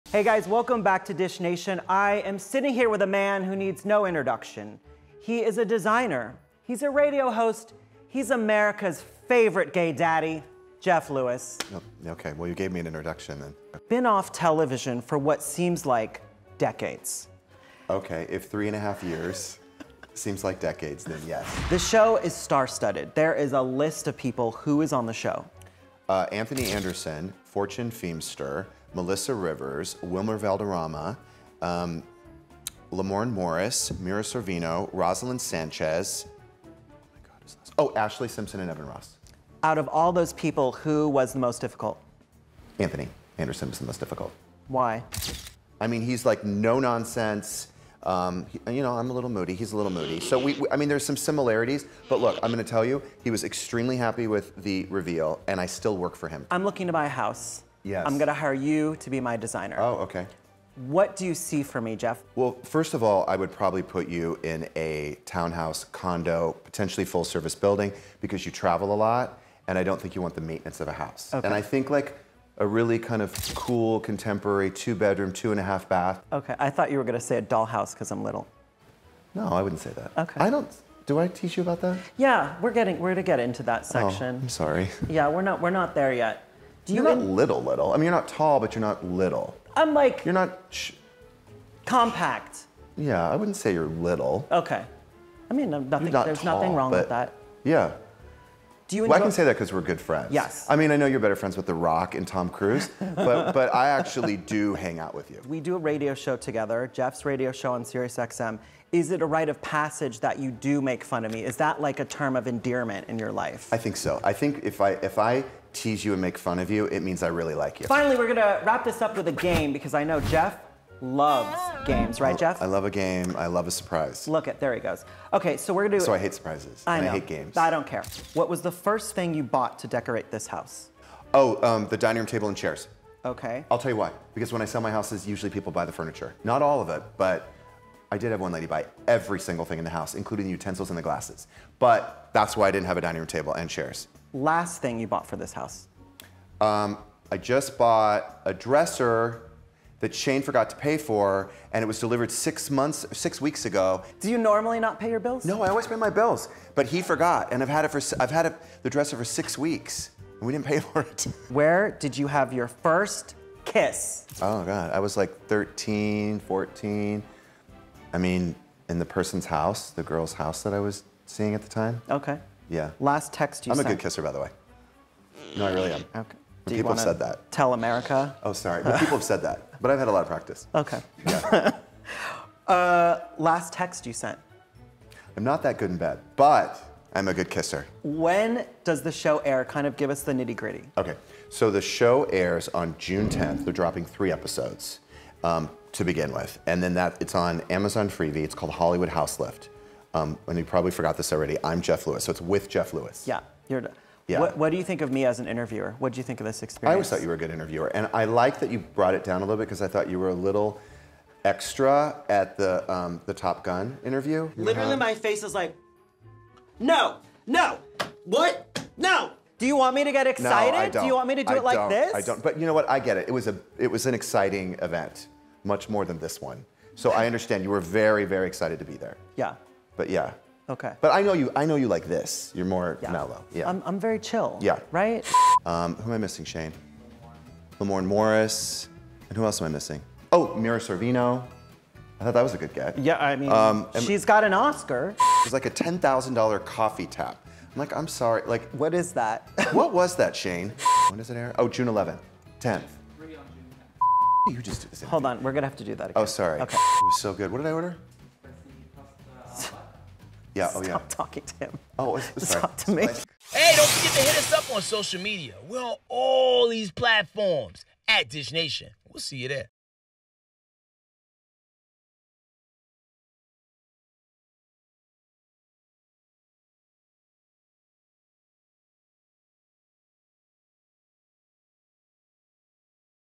Jeff Lewis Dishes On His New Show Hollywood Houselift, The Most Difficult Celebrity Client, & More!
JeffLewis sits down with his pal Andrew Freund to dish on his new show HollywoodHouselift, his most difficult celebrity client, that time he didn't pay a bill, and more! Tune in for the hilarious interview and stream his new show for free on June 10th on freevee!

HOSTS
ATLANTA: